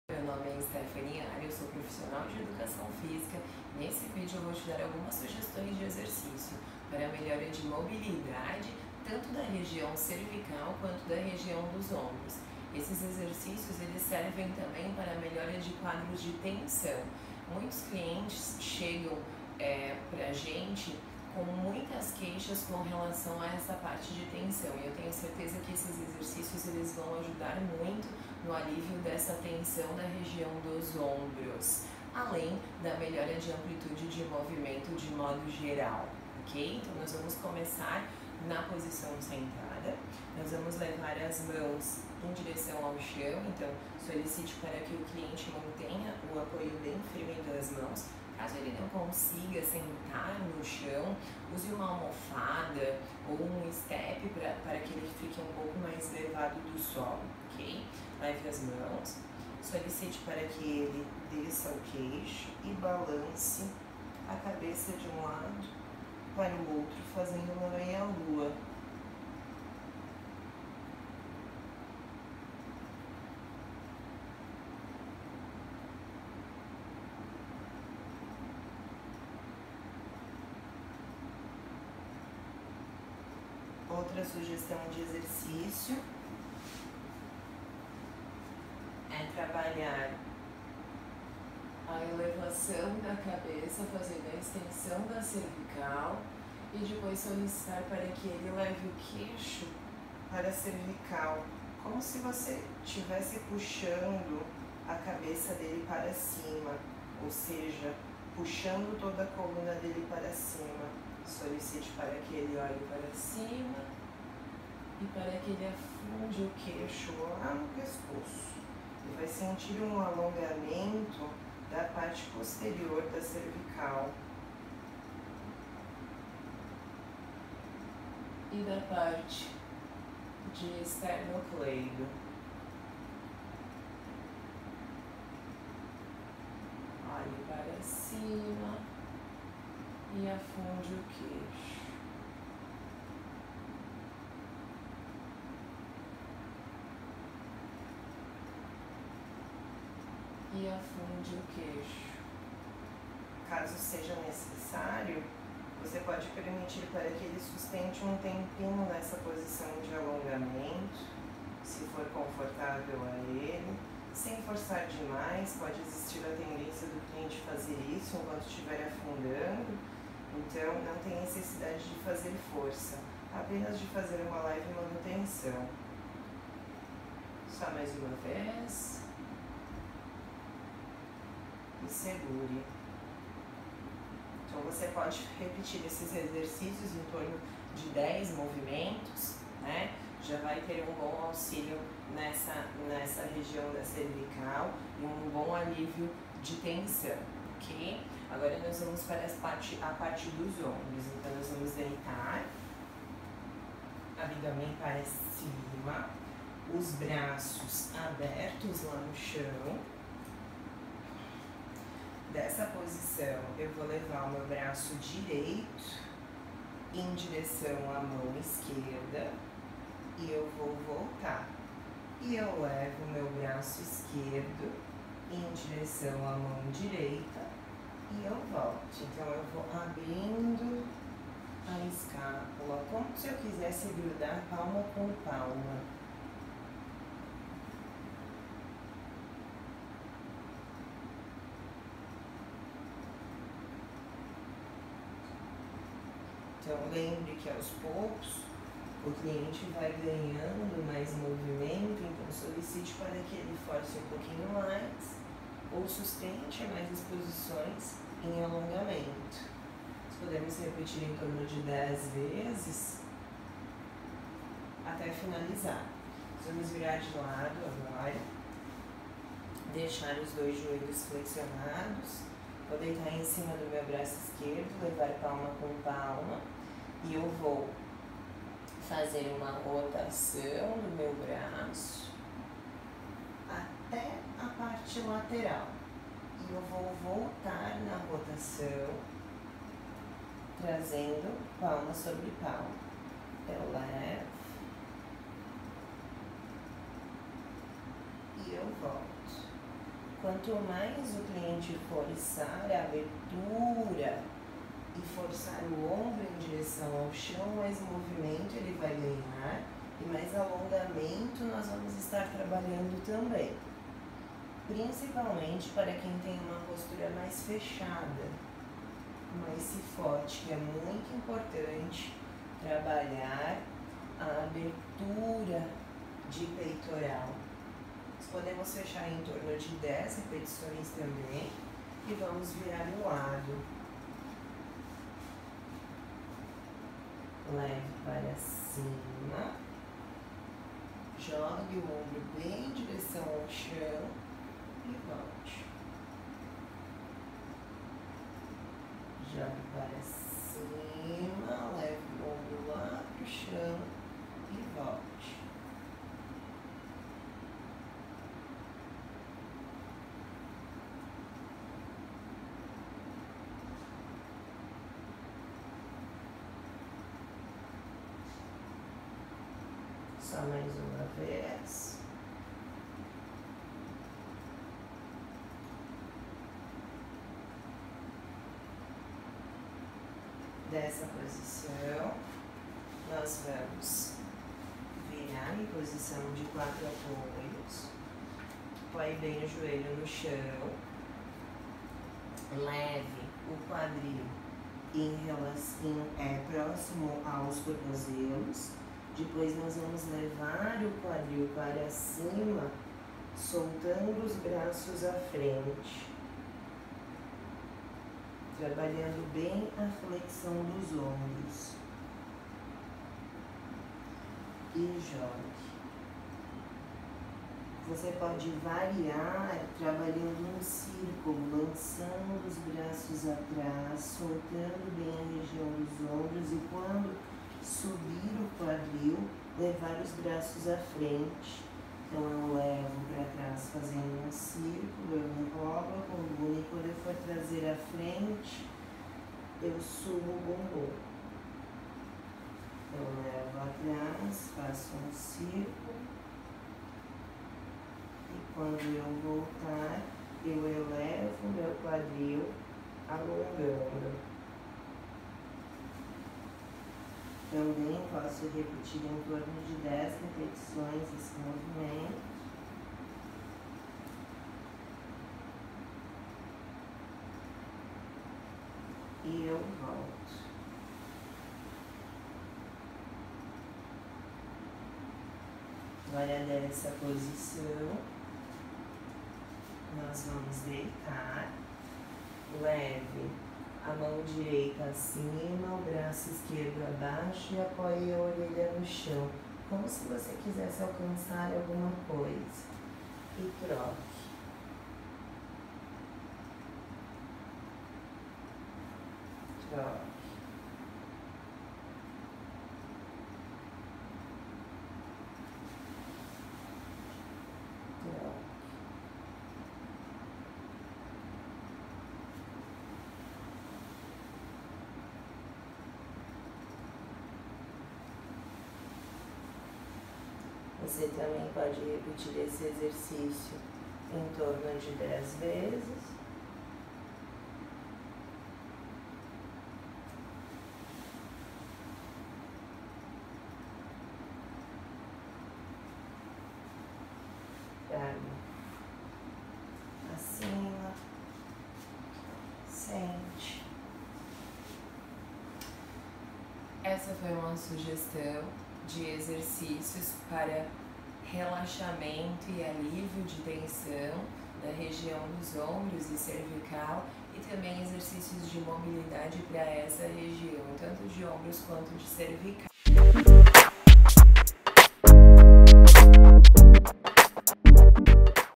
Melhorando a mobilidade de cervical e ombros
Sugestão de exercícios para promover alívio de tensão e melhora de mobilidade cervical e dos ombros

Objetivo: demonstrar exercícios de mobilidade e suas correções e aplicações